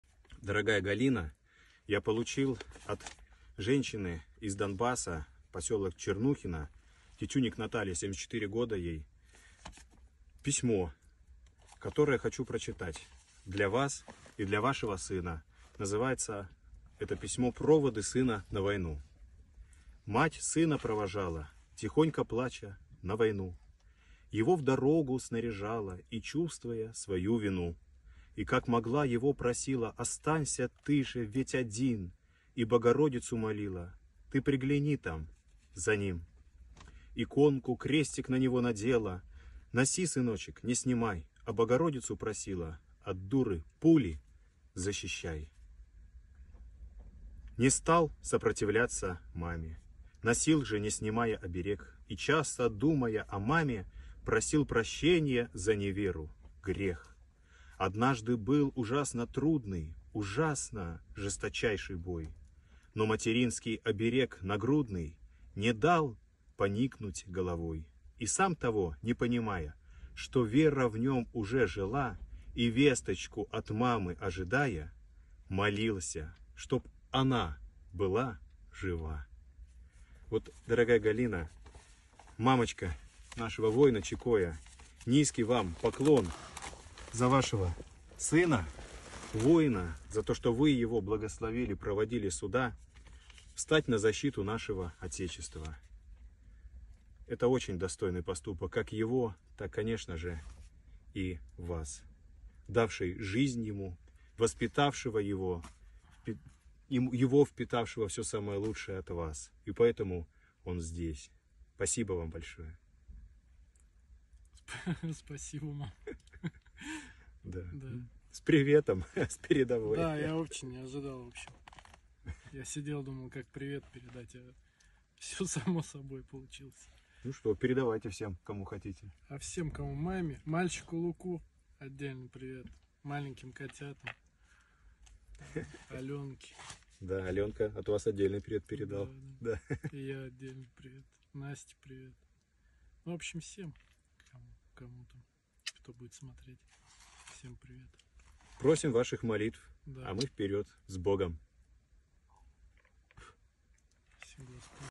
О проводах матери сына на
Приветствую вас, дорогие друзья!
Хочу поделиться с вами очень трогательным моментом.
Совсем недавно получил от Натальи из Донбасса (ей 74 года) письмо со стихотворением о проводах матерью сына на войну.

Так получилось, что после моего воззвания о подъеме русских людей на защиту Отечества, Петр решил пойти служить именно в наше подразделение - Казачий батальон «СКИФ». Вскоре, я познакомился и с его заботливой мамочкой.

Придя в окоп, где несет службу Петр, я решил прочитать это стихотворение с посвящением ему и его мамочке.

Также, Петр передал приветы. Кому - смотрите в этом видео. 😉